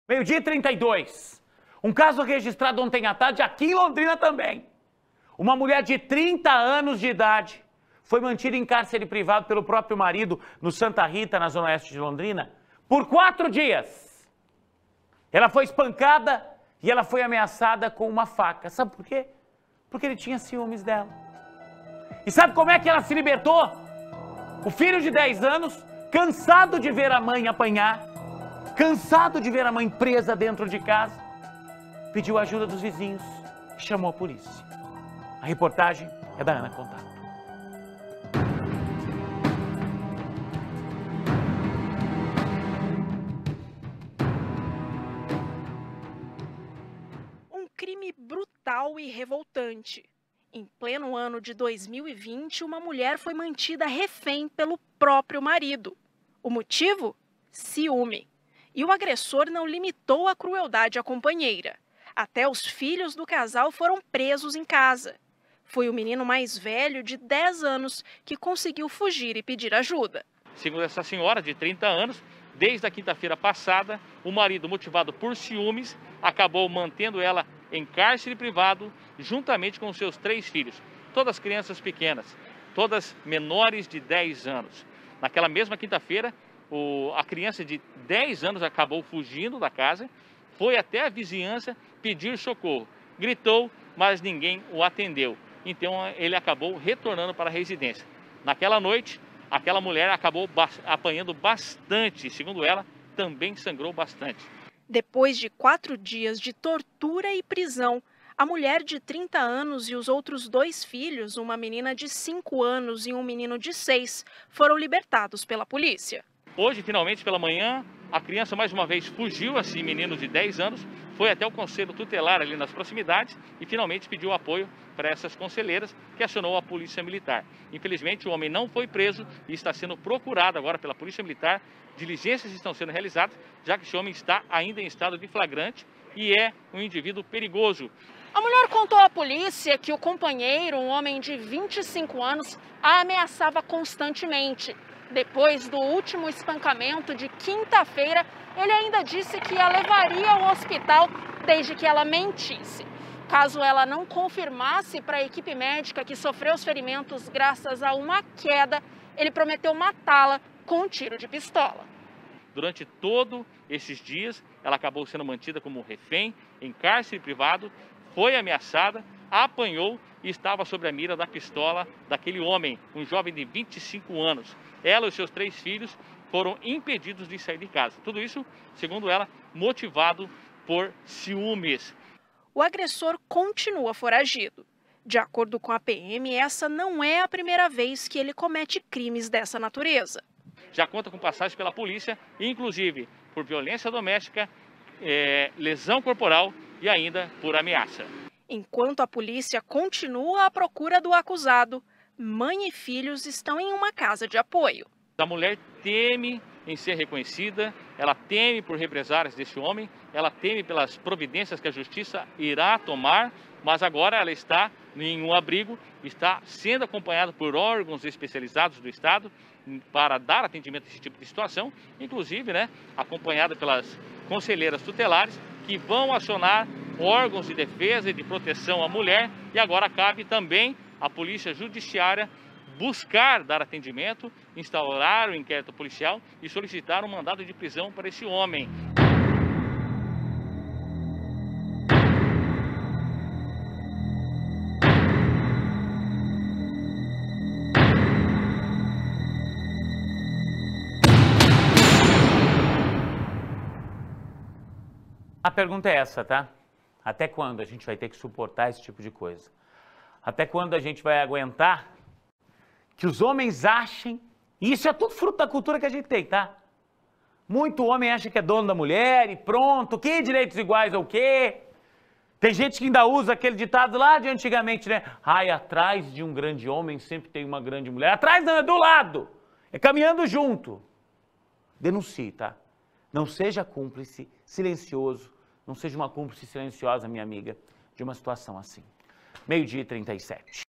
Mulher era mantida em cárcere privado na zona oeste de Londrina
Uma mulher de 30 anos era mantida em cárcere privado pelo próprio marido, no jardim Santa Rita, zona oeste de Londrina. Ela foi espancada todos os dias e ameaçada com uma arma. Confira!